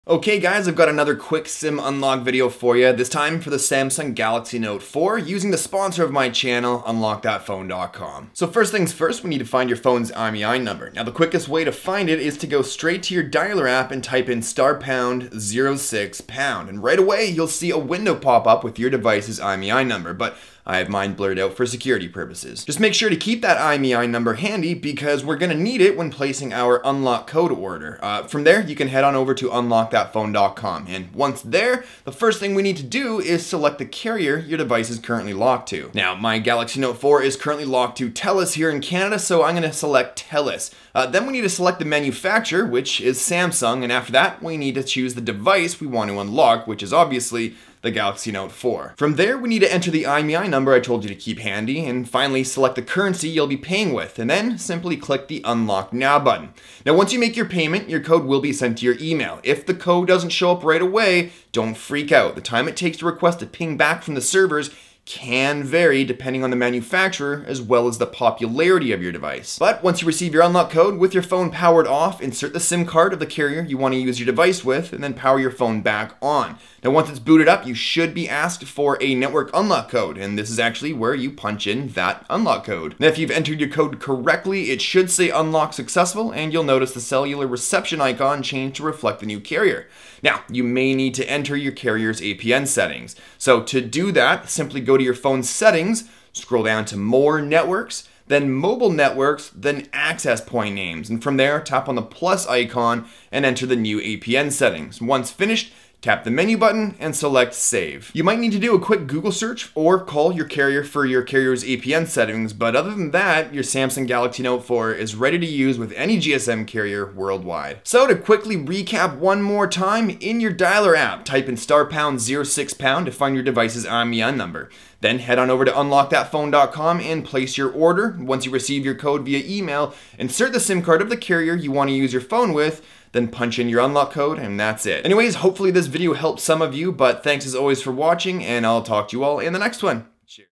How To SIM Unlock Samsung Galaxy Note 4
A quick guide to sim unlocking the Samsung Galaxy Note 4 for the sponsor of the channel,